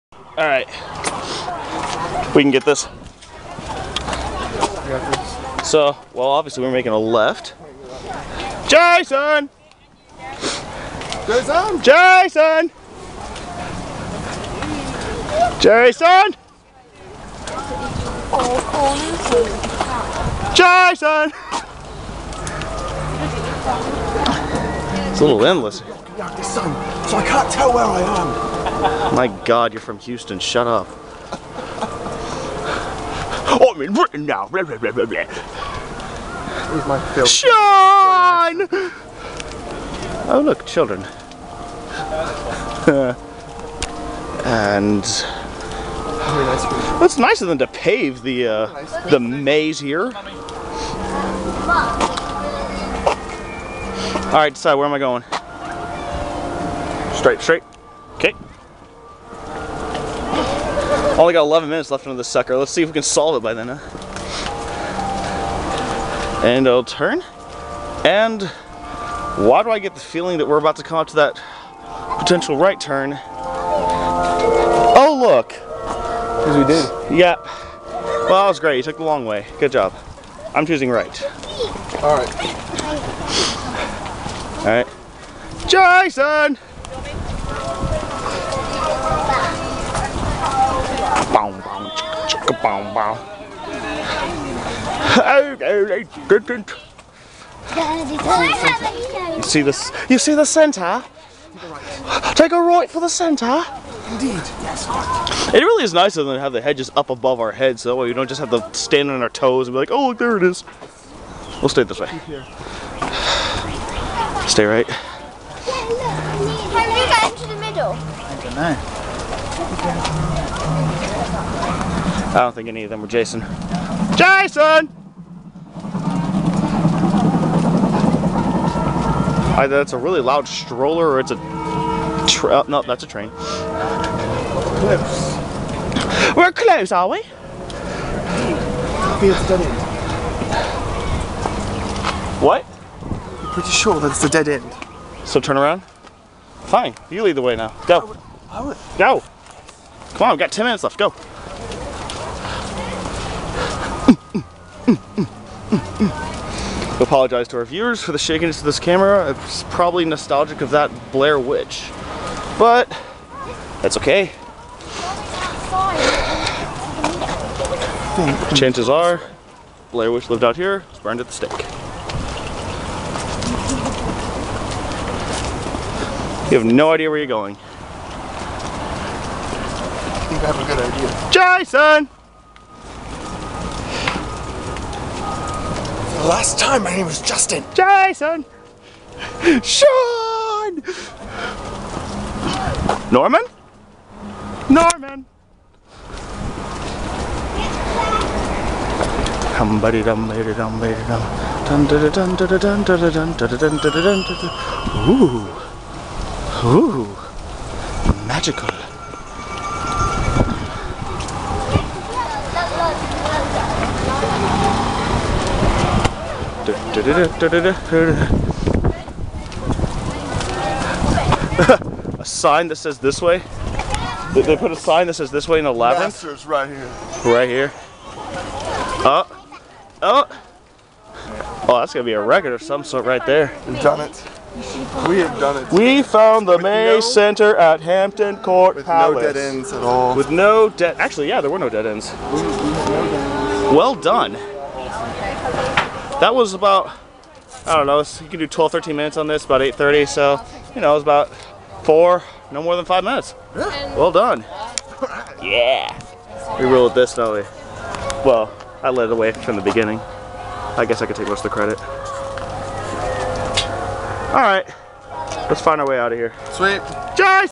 Hampton Court Labyrinth pt. 1
My buddy and I working our way through the hedge maze